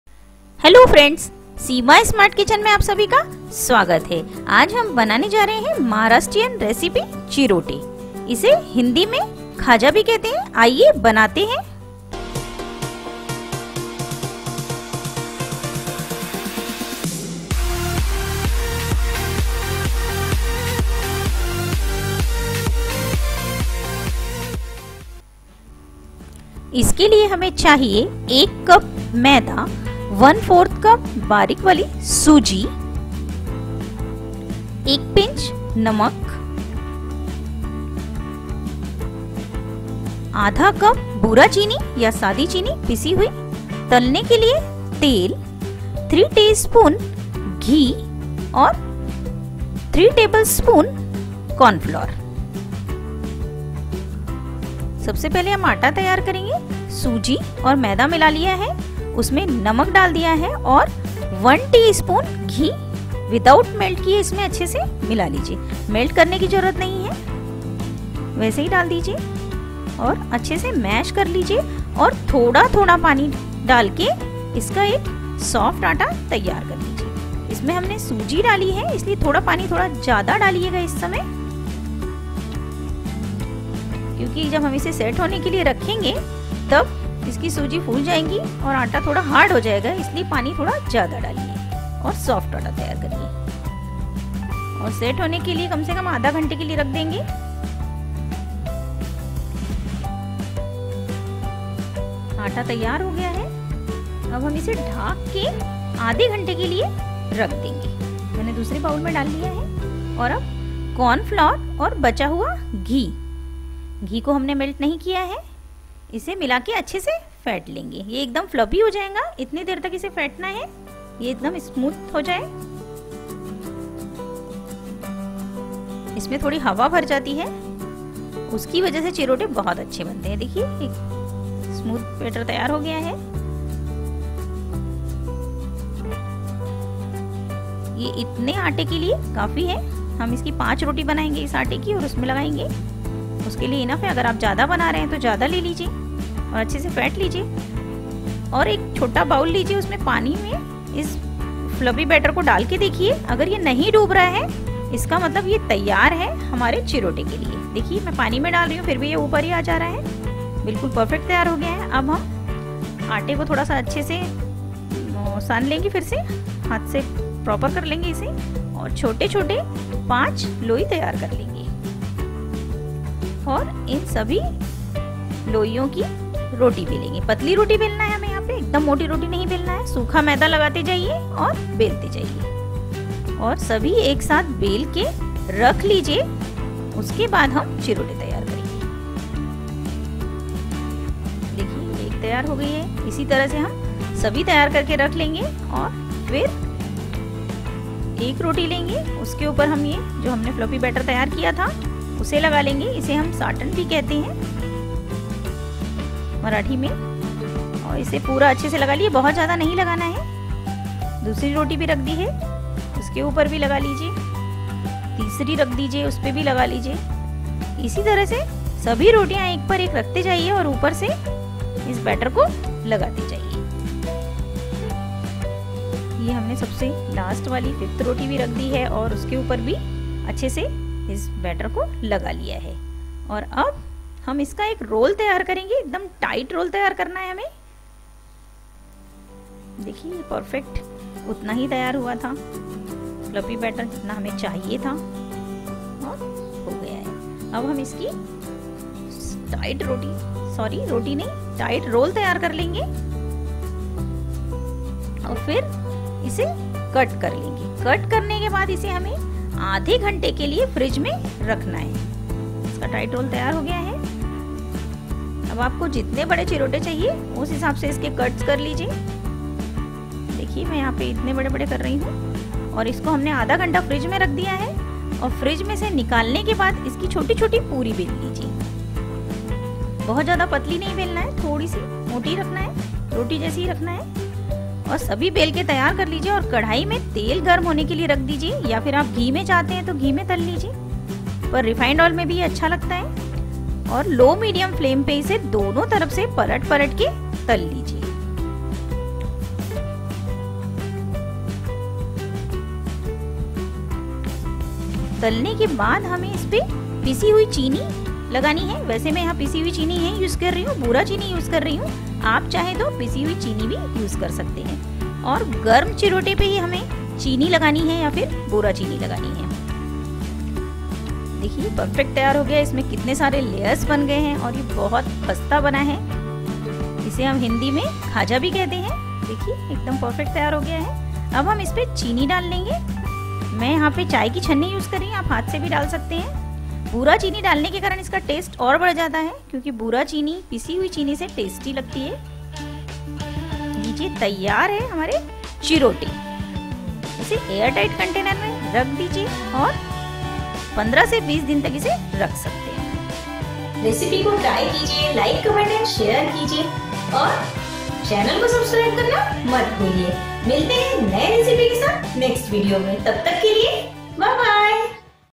होली पर बनाये Chirote - Diwali special recipes
Chirote is a popular Maharashtrian sweet dish. Chirote is very delicious to eat. Here is a step by step and quick recipe of making Chirote. For more recipes like this please visit ans S U B S C R I B E my channel